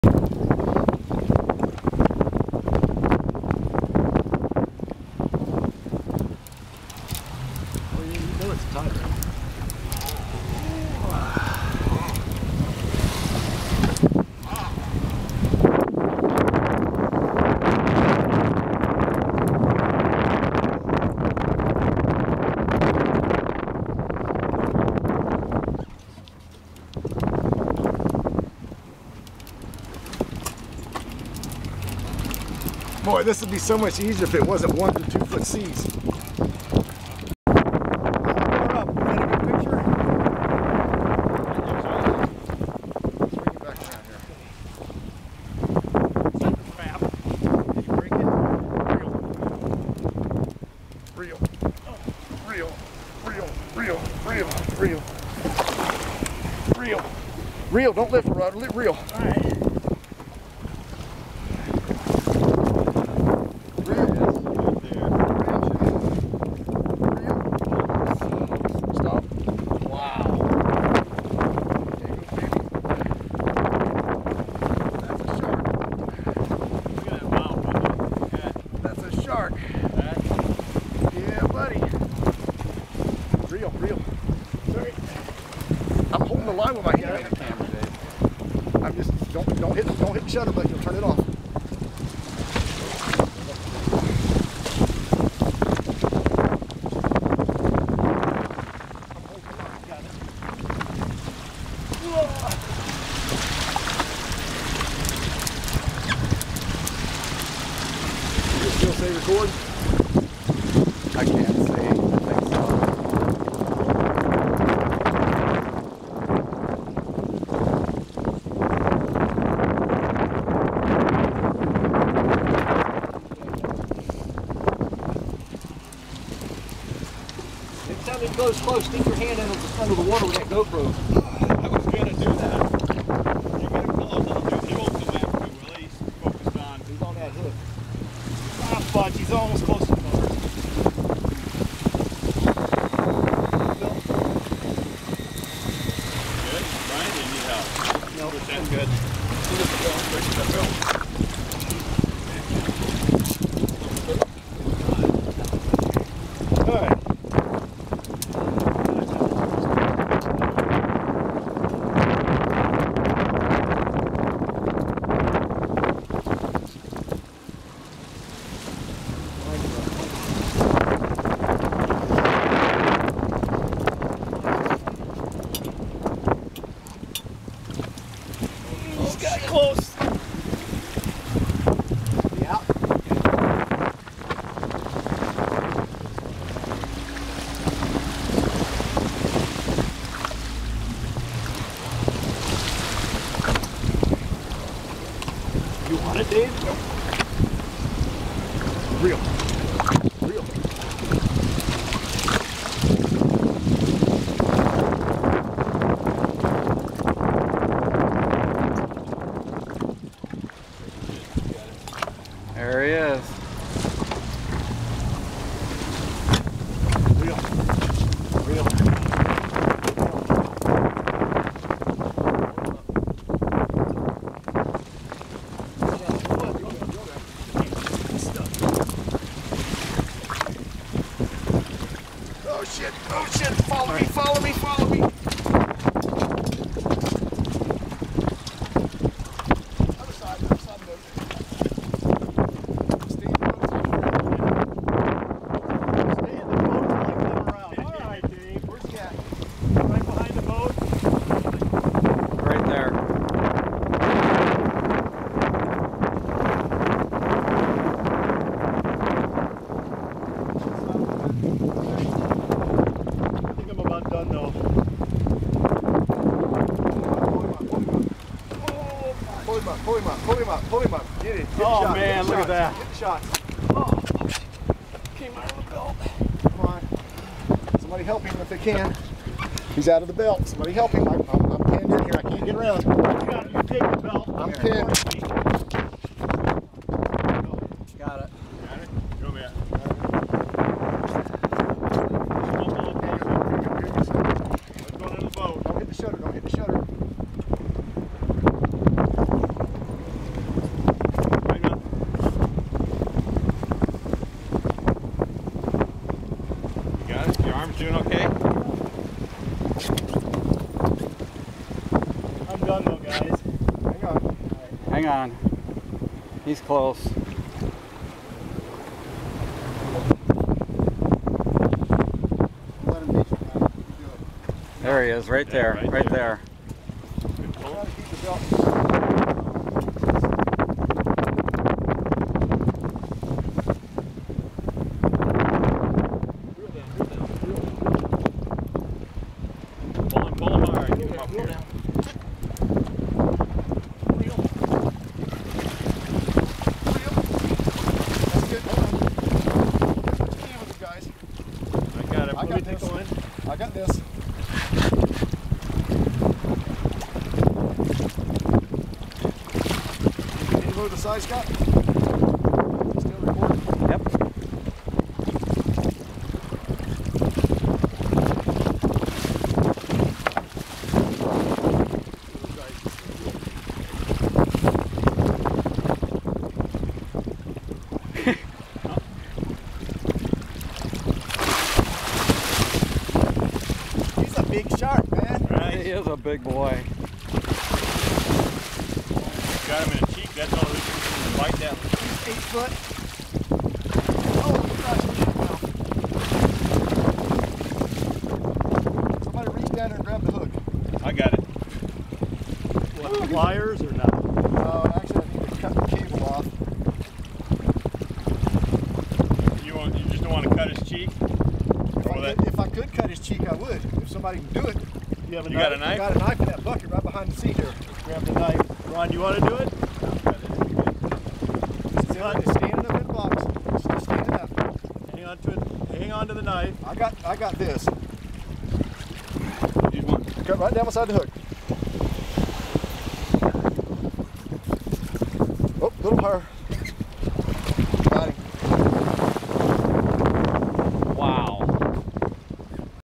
Fishbuster Charters Tiger Shark 5-1-2014
First off the shark was released unharmed. We cut the hook from the skin on its cheek and he swam away, Brian Bettini was on the rod when it ate a jack cravelle it took about 45 mins chasing it with the boat in 4 foot seas and 20 knot winds. Sorry for the audio, like i said it was windy.